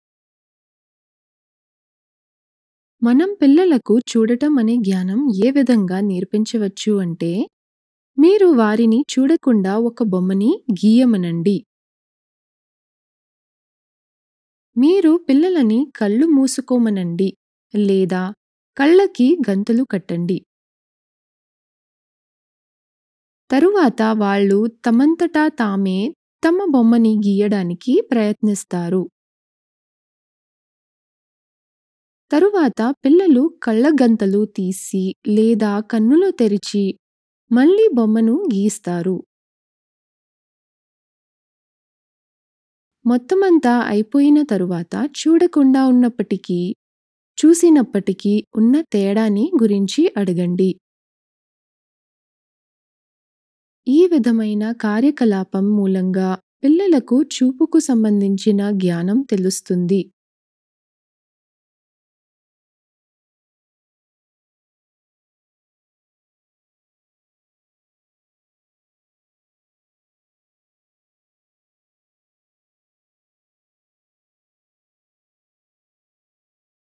చూపుకు సంబందించిన జ్ఞానం - The Sense of Sight (Telugu)
బొమ్మలు గీయటం అనే ప్రక్రియతో పిల్లలకు చూపుకు సంబందించిన జ్ఞ్యానం తెలియజేయవచ్చు.బొమ్మను గీసే టప్పుడు  పిల్లల కళ్ళకు గంతలు కట్టవచ్చు లేదా వారిని కళ్ళు మూసుకోమని చెప్పవచ్చు. పిల్లలు వారి బొమ్మని వారే చూడకుండా ఒక  సారి ,చూసి ఒక సారి గీస్తారు.

జీవితం కోసం నేర్పడం అనే ఈ ప్రపంచ ఉద్యమం లో, మన పిల్లలకు మంచి విద్యను అందిస్తూ మరియు ఉన్నత భవిష్యత్తు ని ఆశిస్తూ అందరూ తమ జ్ఞానాన్ని పంచుతున్నారు.

Telugu translation by Devi Gandikota and Kavitha Madanambedu. Narration by Vineeta Verma.

You can help a child learn about the sense of sight with this drawing activity. You will need a blindfold, or you can ask the child to keep their eyes closed while they draw. The child tries to draw a picture of themselves without being able to see, and then again with their eyes open.